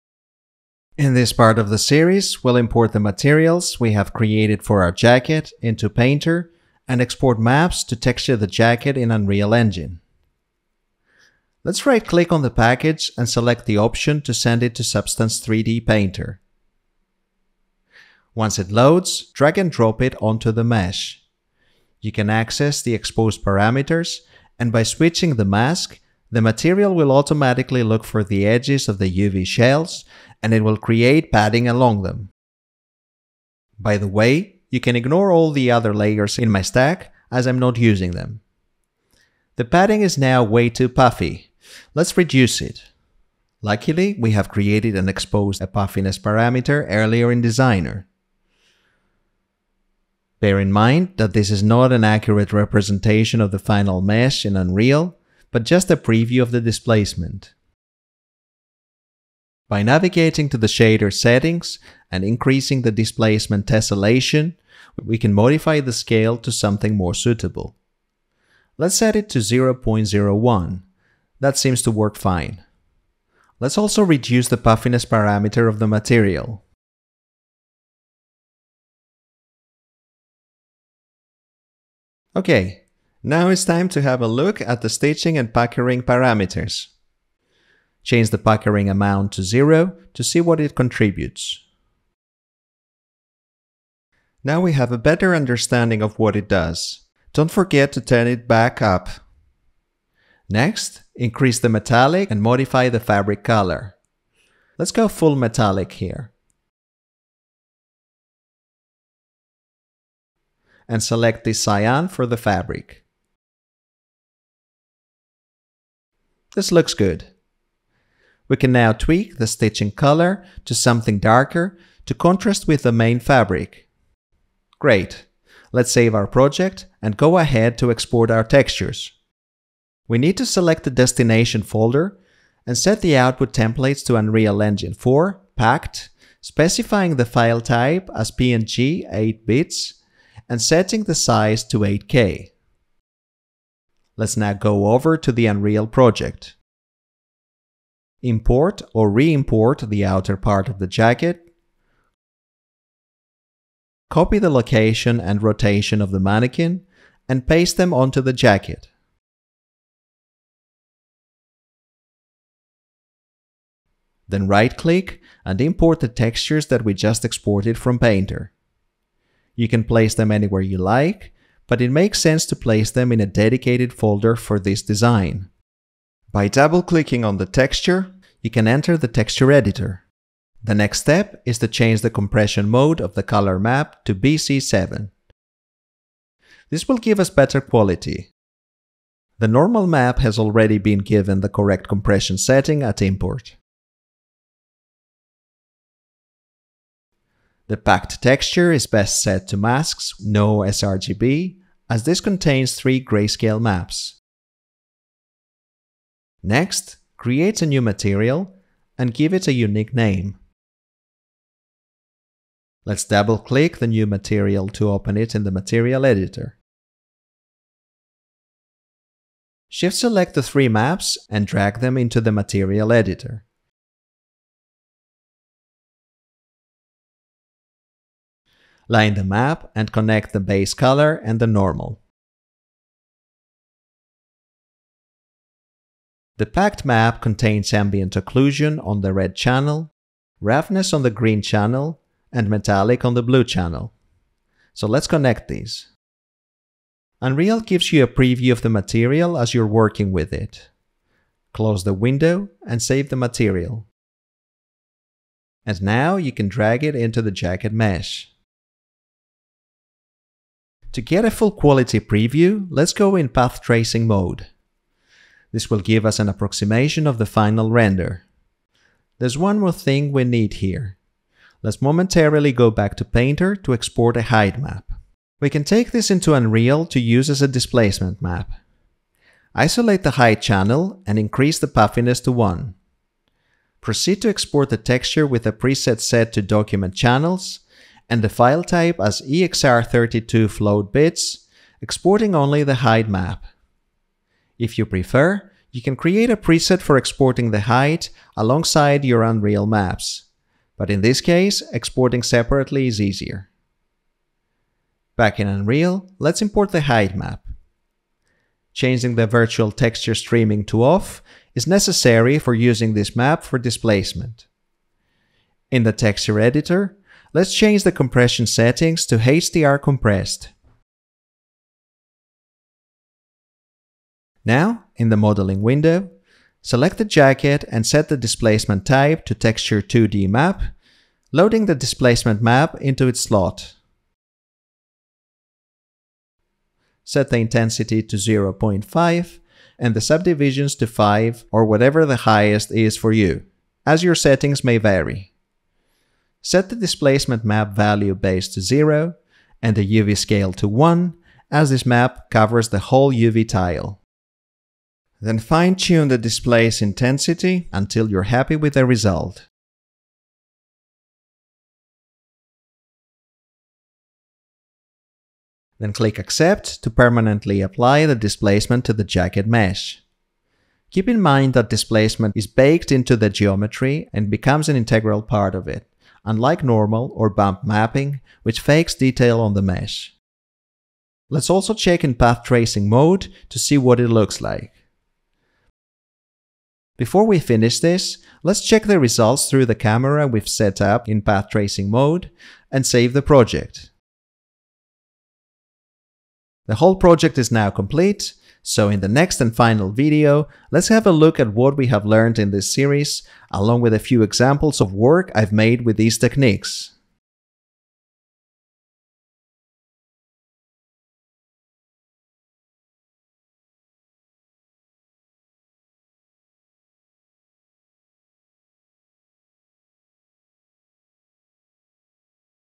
Import into Substance 3D Painter & Export into Unreal Engine: Jacket (Pt 7) | Substance 3D x Clo3D
In this episode, you will learn how to import, enhance and optimize materials in Substance Painter to texture garments, before importing maps from Substance Painter to texture your project in Unreal Engine.

Import into Substance 3D Painter & Export into Unreal Engine: Jacket (Ep. 7) | Substance 3D x Clo3D